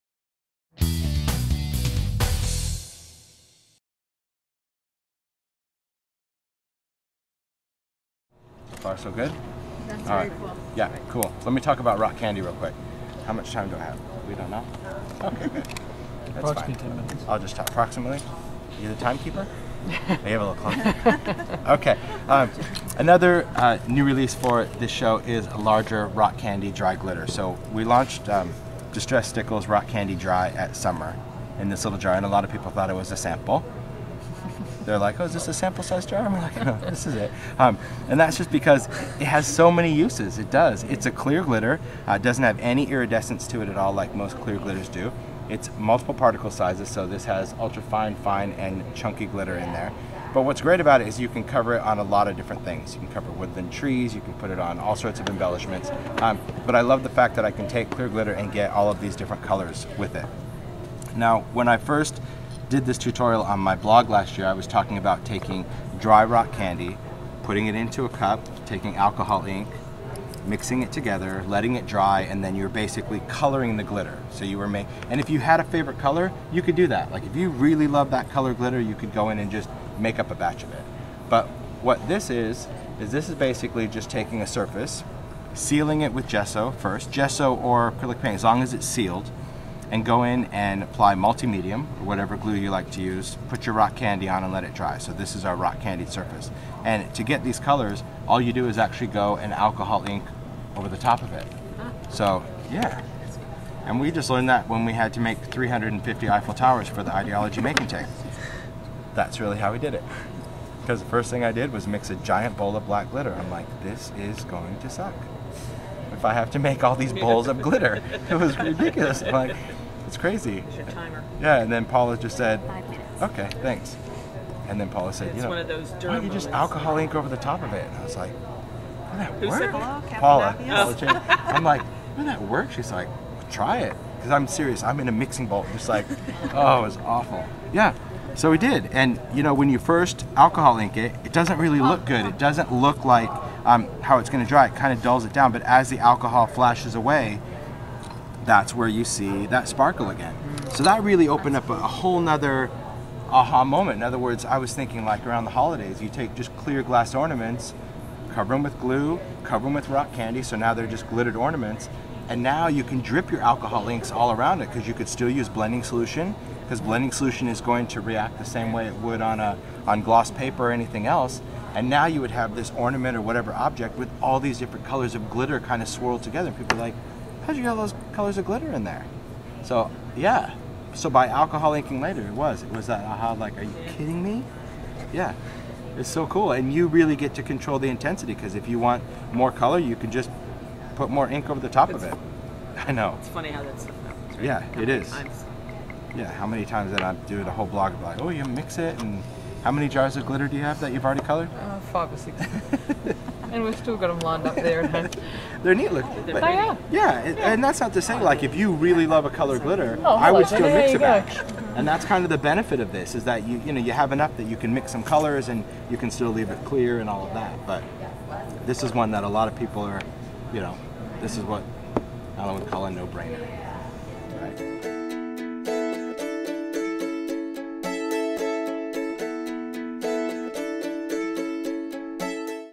Scrap Time - Dyeing Rock Candy Glitter with Tim Holtz
Tim Holtz shows us how to dye Rock Candy Glitter